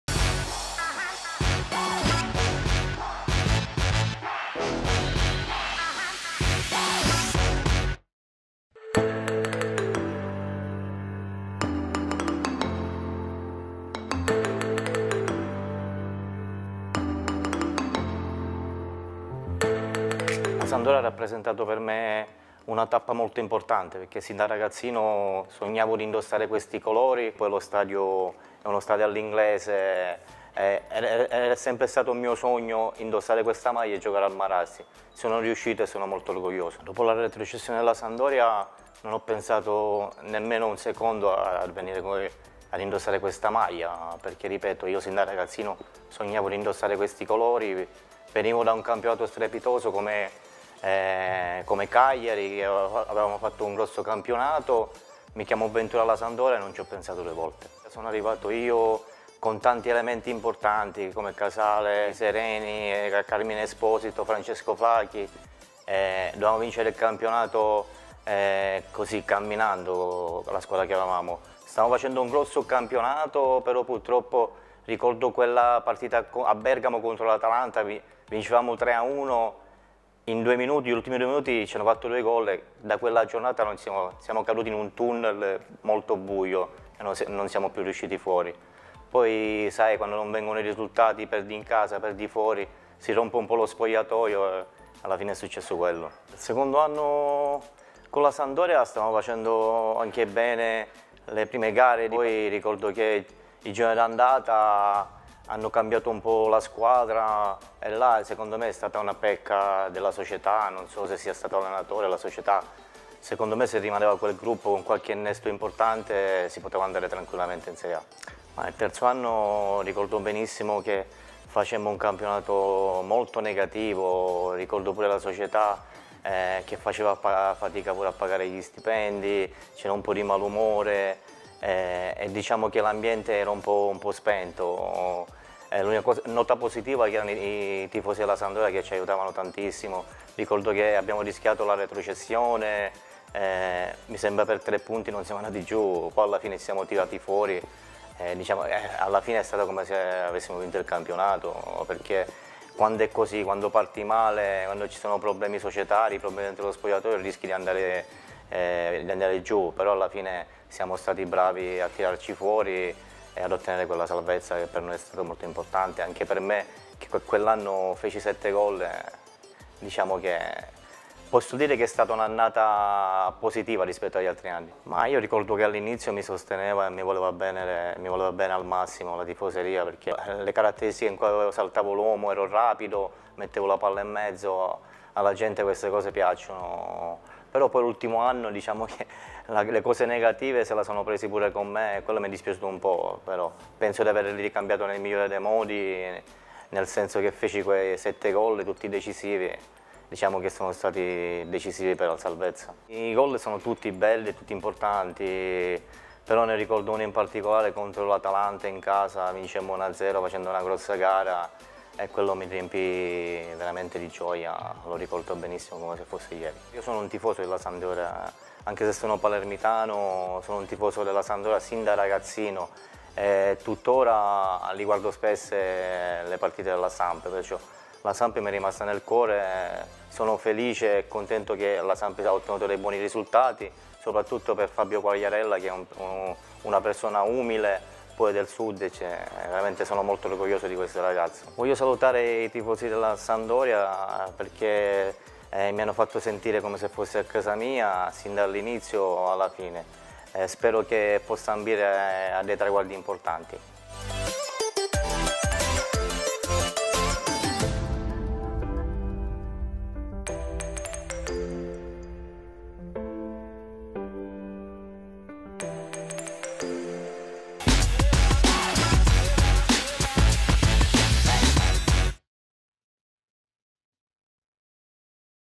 Vasari e il blucerchiato: «Samp, un sogno realizzato»
Un desiderio divenuto realtà. Questo rappresenta la Sampdoria per Gaetano Vasari, che in blucerchiato ha giocato dal 1999 al 2002. «Arrivavo da un campionato strepitoso in Serie A con il Cagliari - ricorda l'ex ala palermitana -, ma non impiegai nemmeno un secondo ad accettare la proposta della Samp nonostante la retrocessione in Serie B. Era una grande squadra avremmo dovuto vincere il campionato a mani basse e invece per due stagioni consecutive sfiorammo la promozione. Mi considero un tifoso: ho realizzato il sogno che avevo da ragazzino di giocare al"Ferraris" con questa maglia e tutt'ora seguo la Samp. Spero che la gente possa tornare a gioire per traguardi importanti».